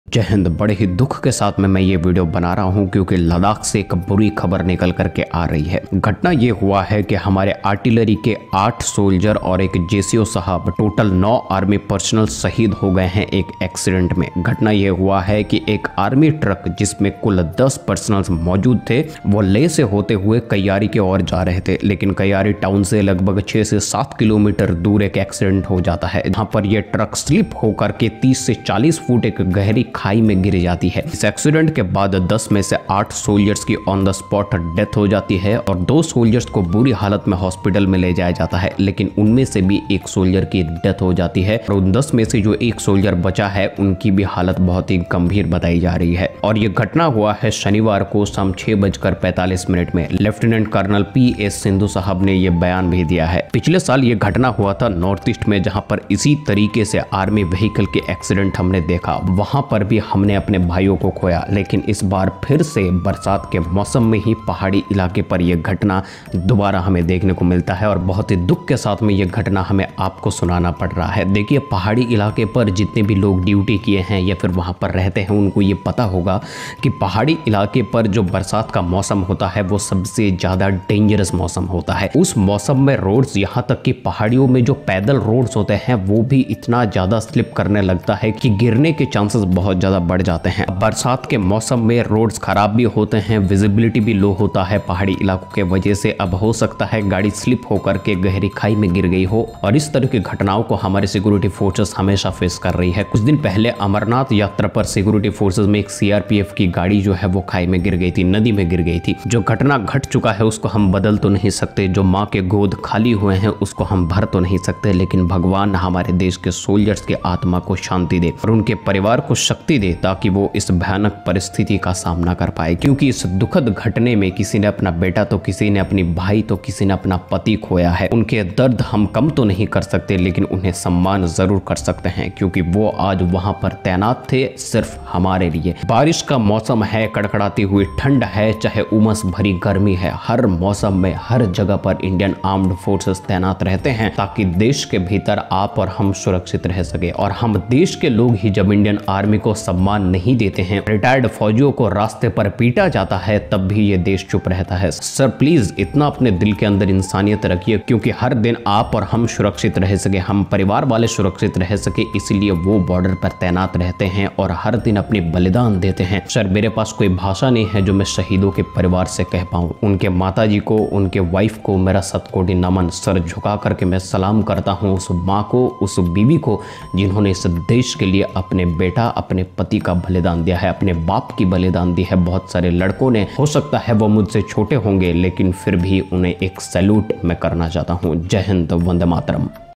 Sad News : Army Truck Accident in Ladakh | We Lost 8 Brave Soldiers & 1 JCO
Army Truck Accident in Ladakh | We Lost 8 Brave Soldiers & 1 JCO

Officials said nine Indian Army soldiers lost their lives in an accident 7 km short of Kiari town. The Army said the vehicle, with soldiers onboard, fell in a gorge. “Many others were injured in the incident,” the Army said.

The vehicle was moving from the Karu garrison to Kiari near Leh.

The Army said the soldiers were in an ALS (advanced life support) vehicle, which was moving as part of a convoy from Leh to Nyoma. It skidded off the road into the Valley at around 5.45-6 p.m. IST, near Kiari. There were 10 personnel travelling in the vehicle out of which nine died and one got injured.